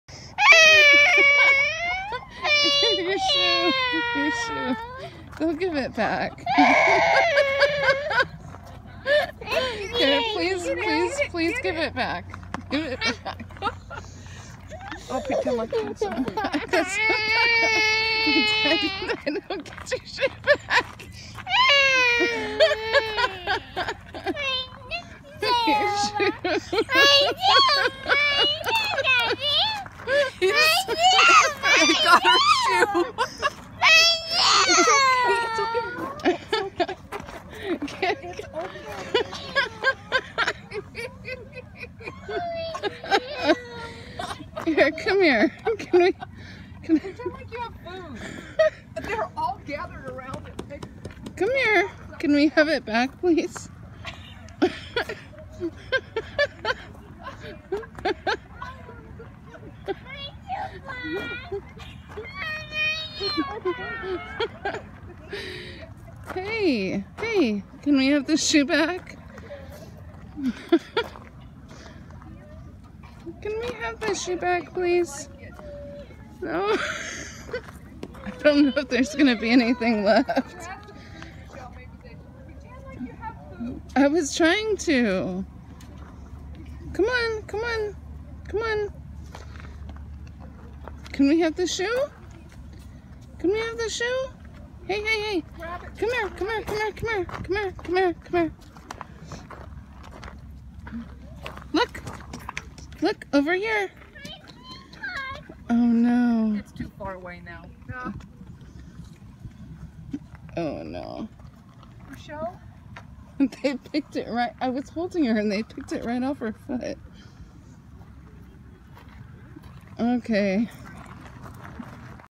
Ostriches Steal Baby’s Shoe Off Her Foot
We were posing for a selfie with Ostriches in the background when they stole Evvies shoe... OstrichlandUSA Santa Ynez, Ca Great time, great story... Thanks to Matthew for rescuing “mine shoe!” 🥰